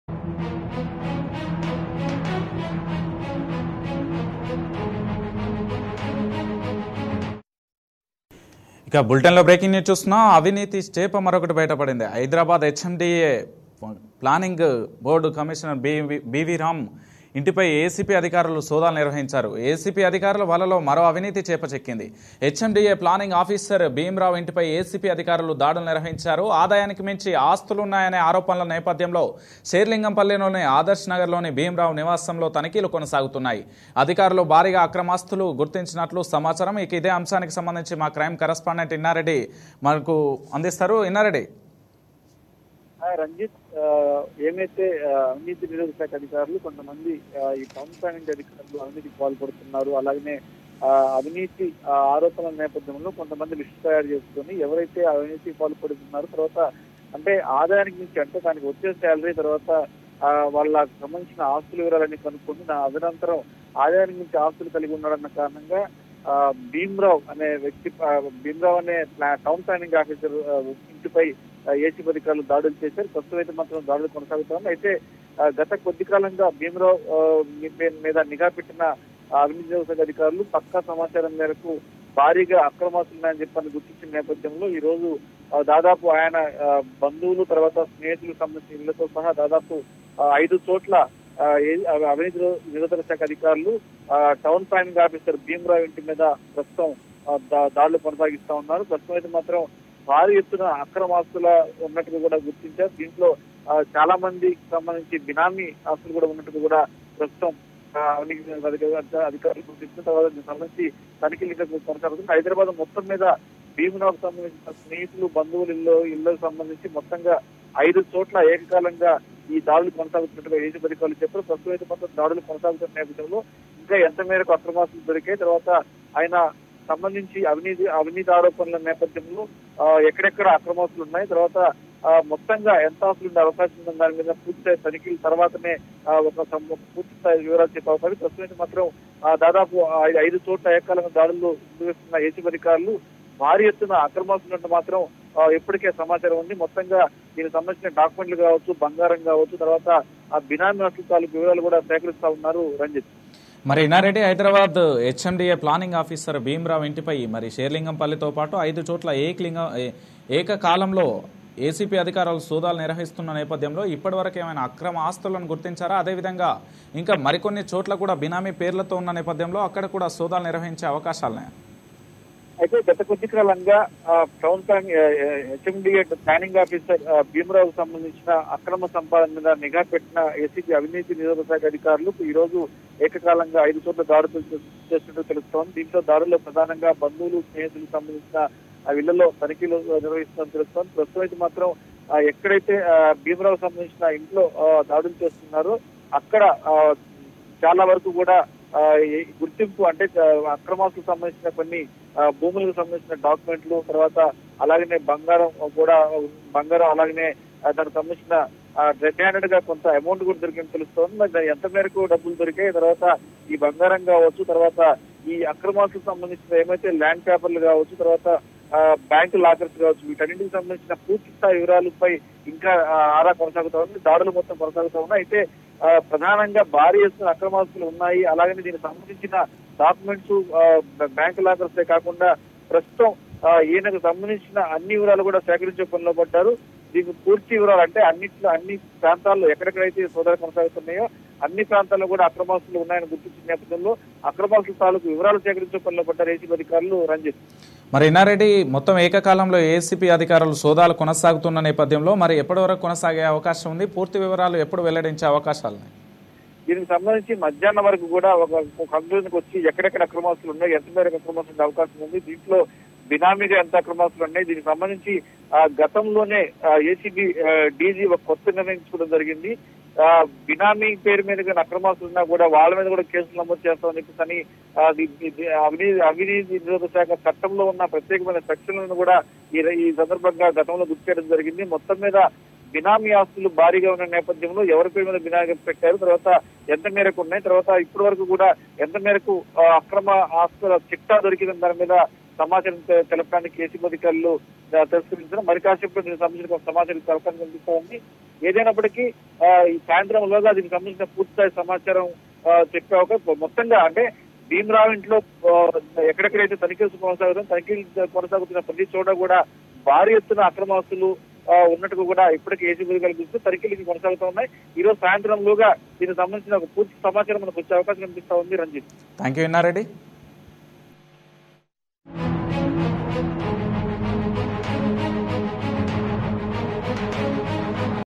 Acb Rides on HMDA Planing Officer Bheem Rao House
𝐖𝐚𝐭𝐜𝐡 Acb Rides on HMDA Planing Officer Bheem Rao House

𝐓𝐨𝐩 𝐓𝐫𝐞𝐧𝐝𝐢𝐧𝐠 𝐁𝐫𝐞𝐚𝐤𝐢𝐧𝐠 𝐍𝐞𝐰𝐬 𝐨𝐧 𝟗𝟗𝐓𝐕 𝐋𝐢𝐯𝐞 :

99%'s news is biased - bias not towards a political group but biased in favour of people.